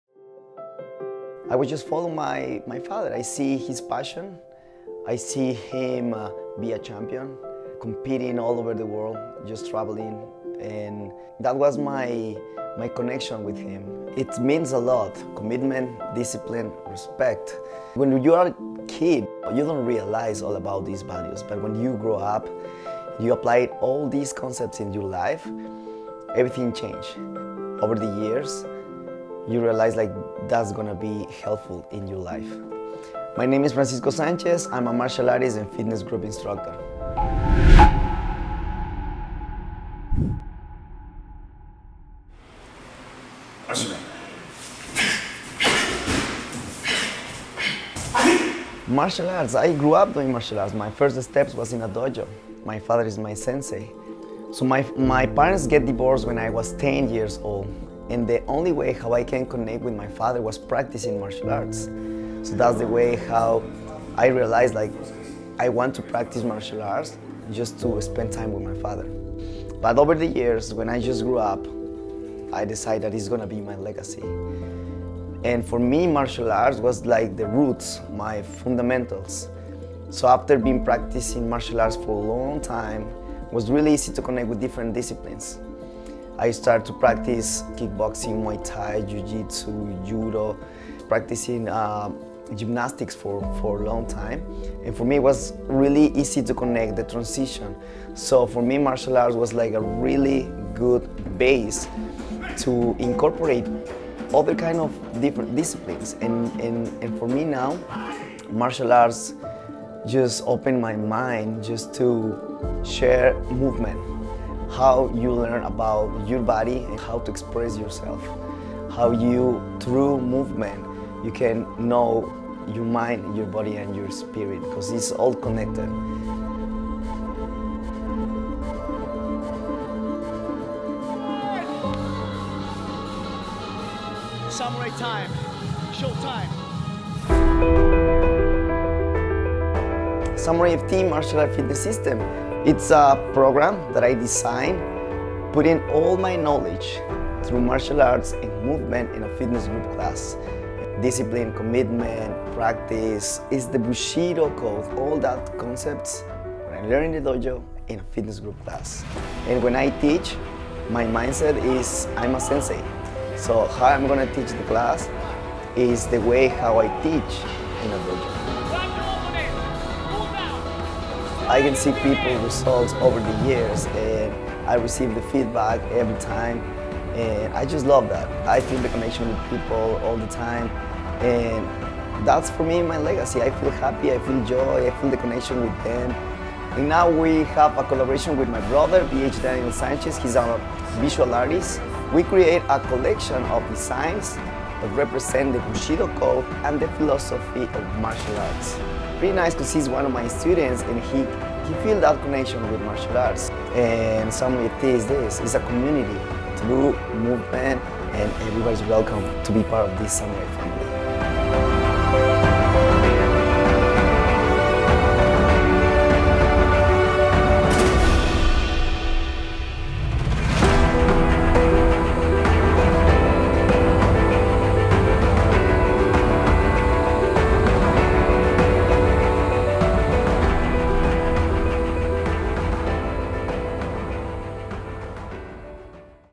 Francisco Sánchez Founder of SamuraiFT Martial Arts Fitness System
Hi everyone! I want to share this mini-documentary with you!!
Finding Purpose Through Martial Arts | Martial Arts Documentary Series
Stealing Fire Originals a Martial Arts Documentary Series. In this episode Francisco Sanchez Martial artist and Founder of SamuraiFT shares his story about finding a purpose in life through martial arts.

Filmed and Produced by:
PH Daniel Sanchez
In Collaboration with:
Francisco Sanchez founder of
SamuraiFT